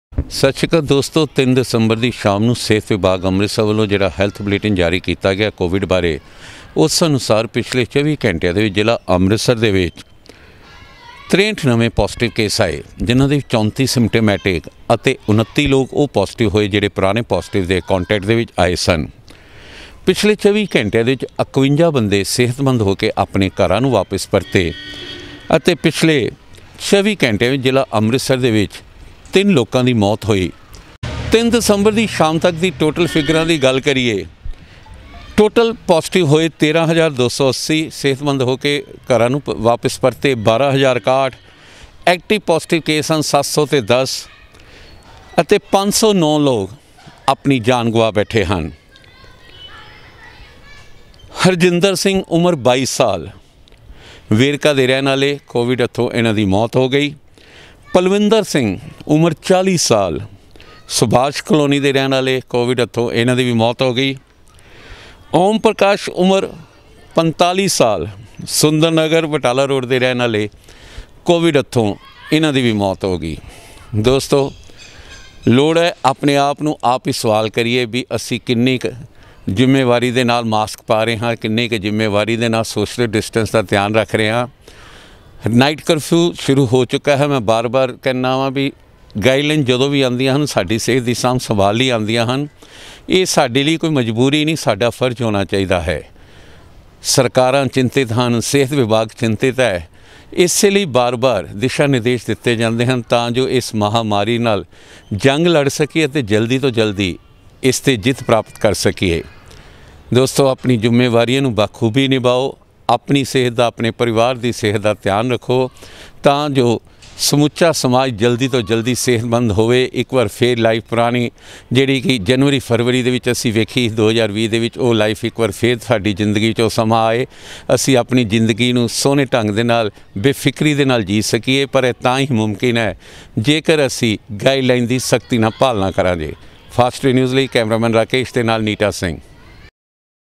63 ਲੋਕਾਂ ਦੀ ਰਿਪੋਰਟ ਆਈ ਕਰੋਨਾ ਪਾਜੀਟਿਵ ਜਿਲ੍ਹਾ ਅੰਮ੍ਰਿਤਸਰ ਵਿੱਚ ਕੁਲ ਐਕਟਿਵ ਕੇਸ 710 3 ਵਿਅਕਤੀ ਦੀ ਕਰੋਨਾ ਨਾਲ ਮੋ
63 ਲੋਕਾਂ ਦੀ ਰਿਪੋਰਟ ਆਈ ਕਰੋਨਾ ਪਾਜੀਟਿਵ ਜਿਲ੍ਹਾ ਅੰਮ੍ਰਿਤਸਰ ਵਿੱਚ ਕੁਲ ਐਕਟਿਵ ਕੇਸ 710
3 ਵਿਅਕਤੀ ਦੀ ਕਰੋਨਾ ਨਾਲ ਮੋਤ ਹੋਈ
 03 Dec 2020